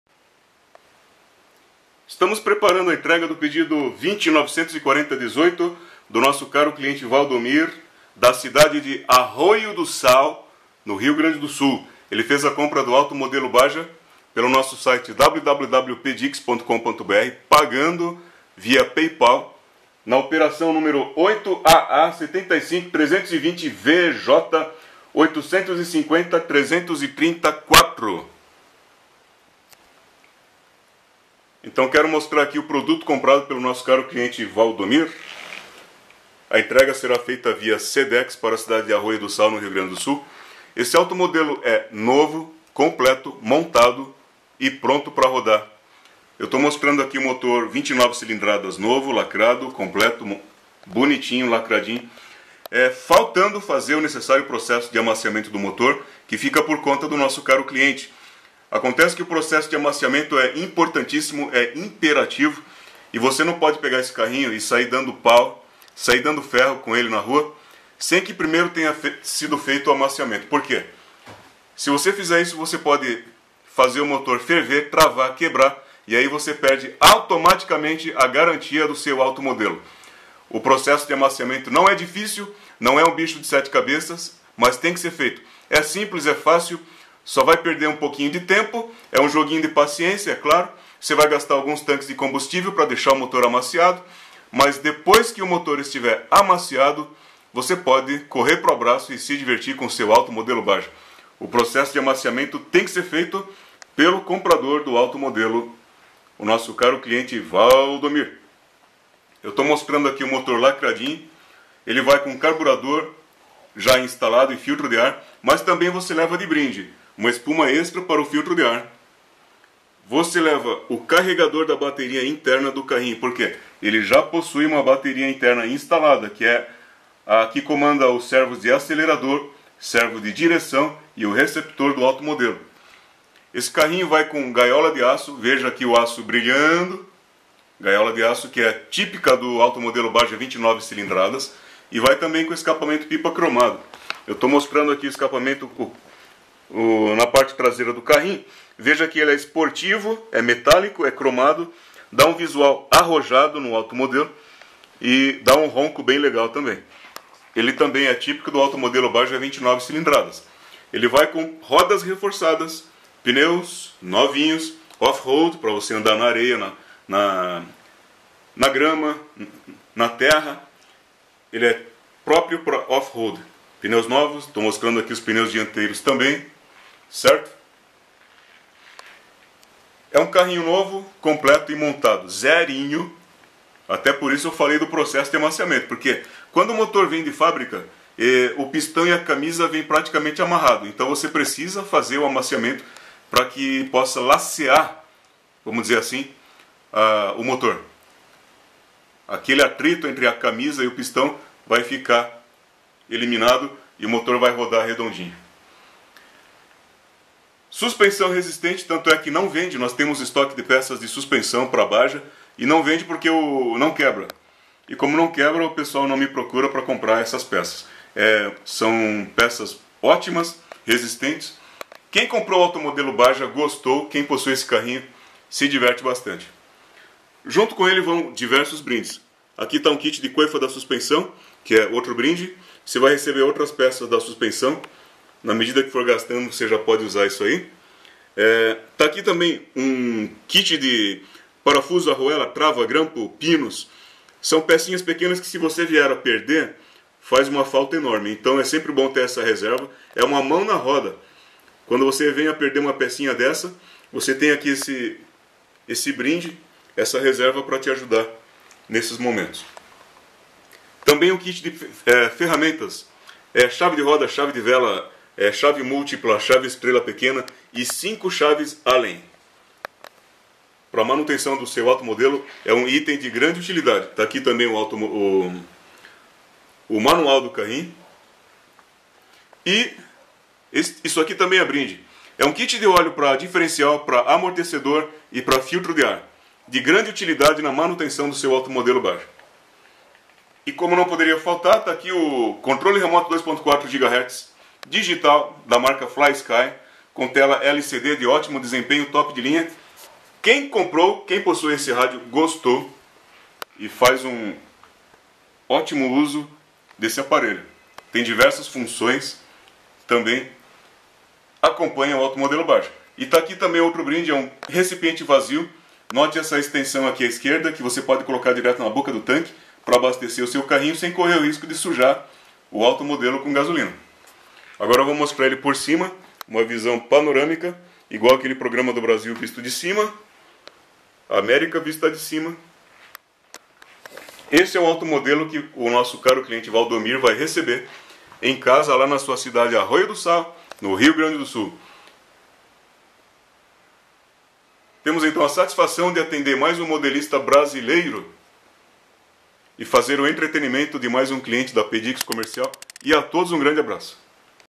COMO AMACIAR MOTOR GASOLINA de Automodelo 1/5 Rovan Baja 29cc NOVO dest Arroio do Sal RS
Estamos preparando a entrega do Pedido 2094018 via Sedex nesta data, para o nosso caro cliente da cidade de Arroio do Sal RS, sendo um Automodelo escala 1/5 modelo Baja com motor 29cc gasolina novo, lacrado, completo e pronto para rodar, faltando fazer somente o necessário processo de amaciamento do motor que fica por conta do comprador, além de incluir um rádio controle remoto 2.4Ghz da marca FlySky digital com tela LCD topo de linha, manual ferramentas e brindes diversos.